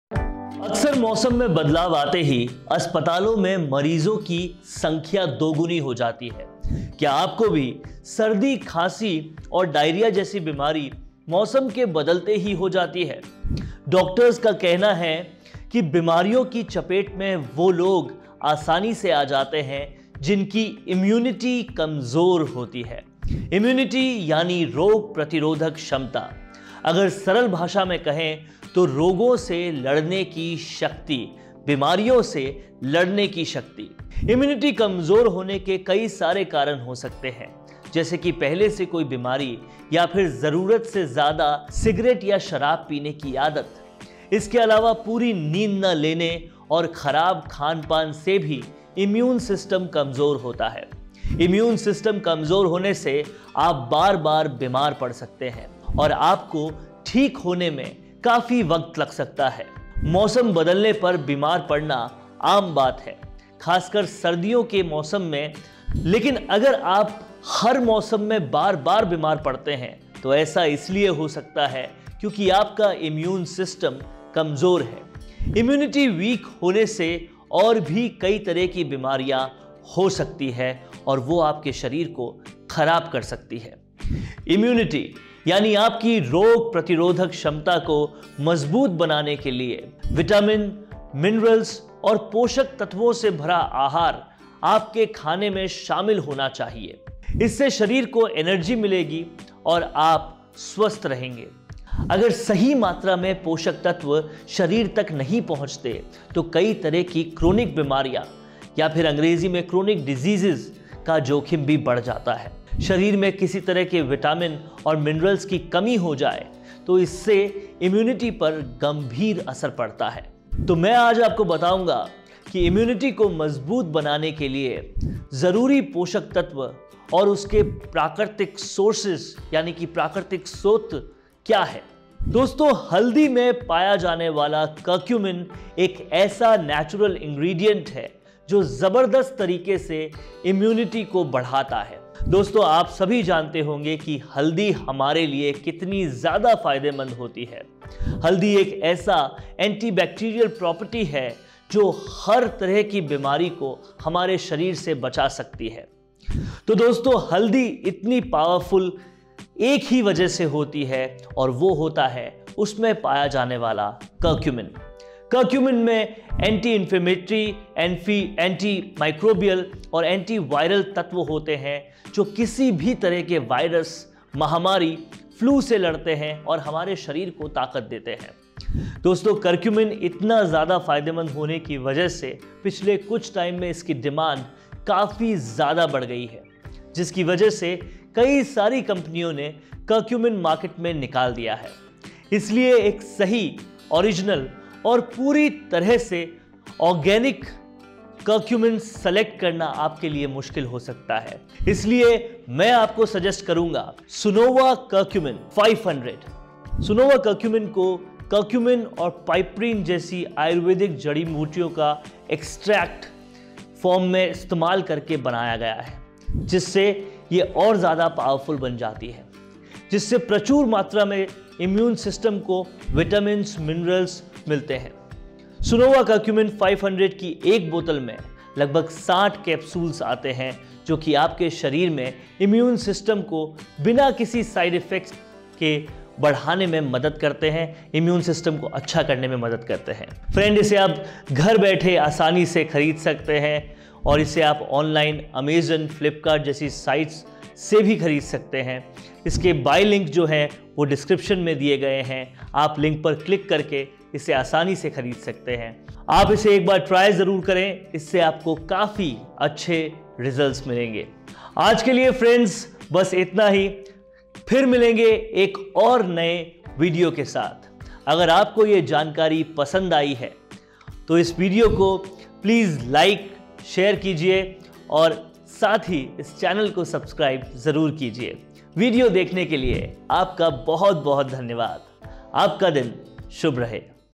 Best Ways to Boost Your Immunity at Home! In This Video I Discussed How To Boost Immunity Naturally
Hello Friends, In this video I discussed how to boosting immunity at how naturally, and i suggested you a best natural product for immunity boosting. Please watch this video till the end for complete information.